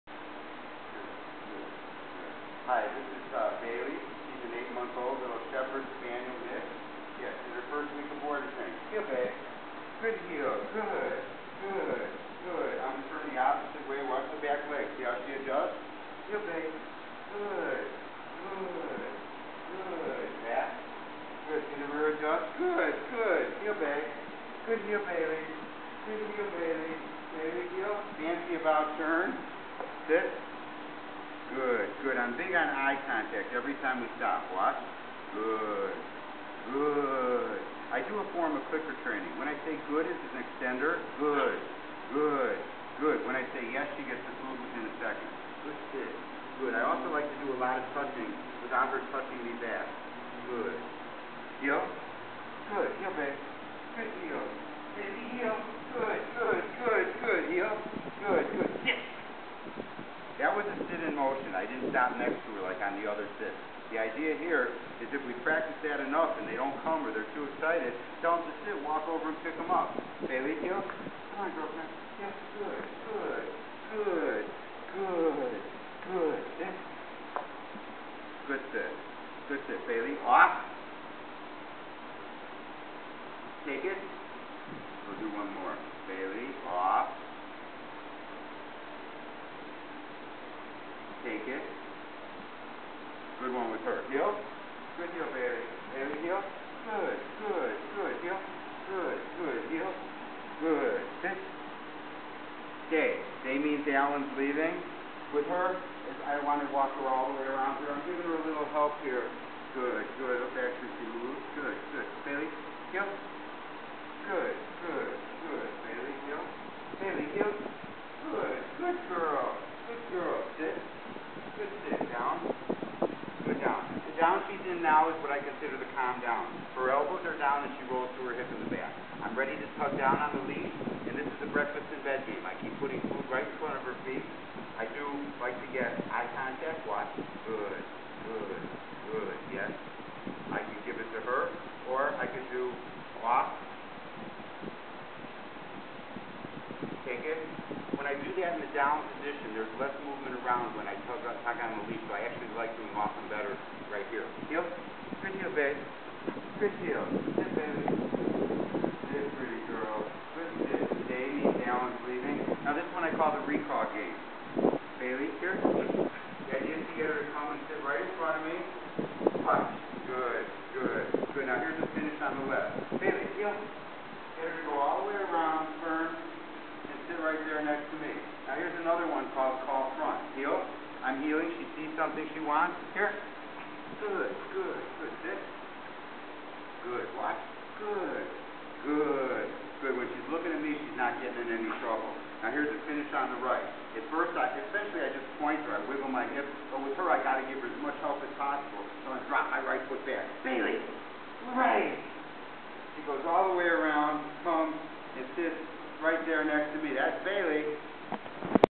Allan Ross Dog Training
Bailey M. is an 8 month American Fox Hound mix in her first week of Board and Train.  We are doing a pattern in the Canine College.  I include a "sit" in motion, the recall game with a finish on the left and a call to front and a finish from my right around to my left.  In the first week we work in a quieter area.  Perfecting is taking what she has learned and duplicating that in more stimulating areas.